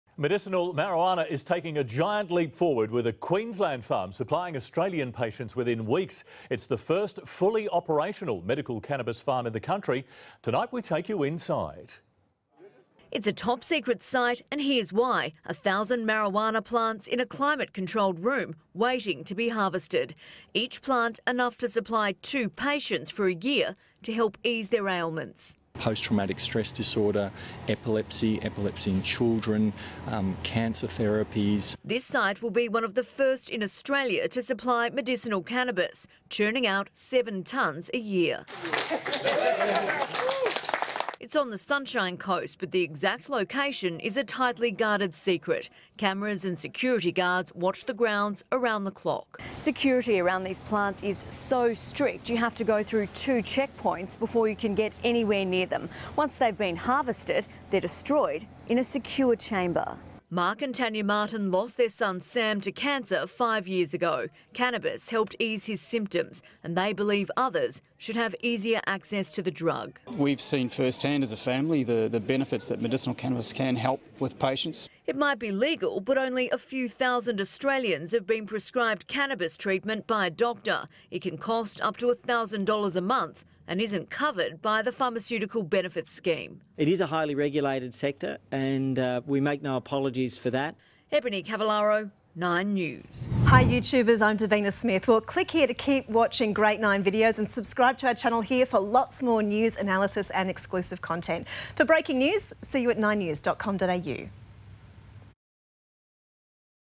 Inside Australia’s first fully operational medical cannabis farm | Nine News Australia
Medicinal cannabis has taken a giant leap forward with a Queensland farm supplying Australian patients within weeks. Nine News goes inside the first fully operational medical cannabis farm in the country.